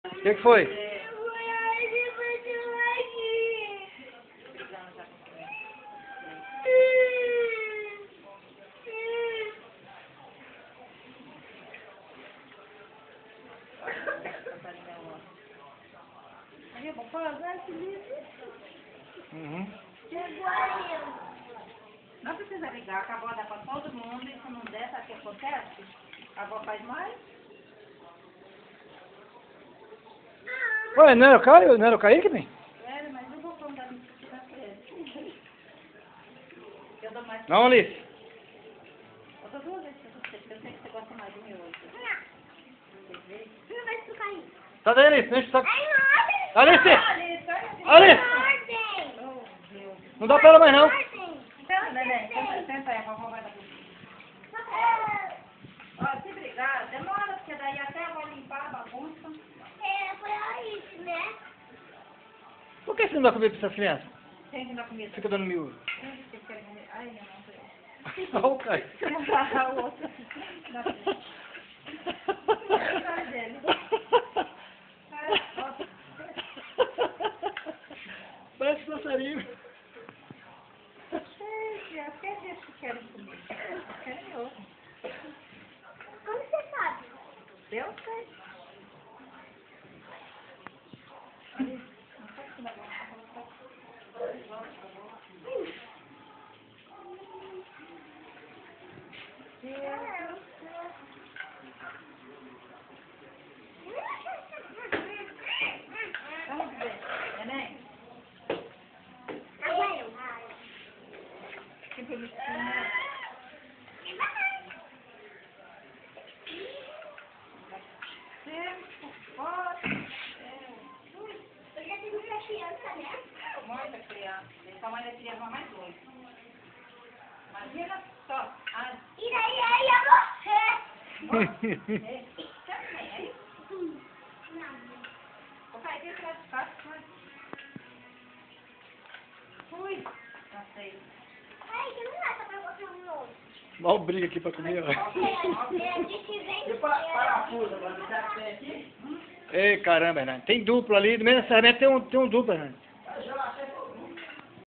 Comendo miojo
Alice, Kaick e Caio disputando uma panela de miojo.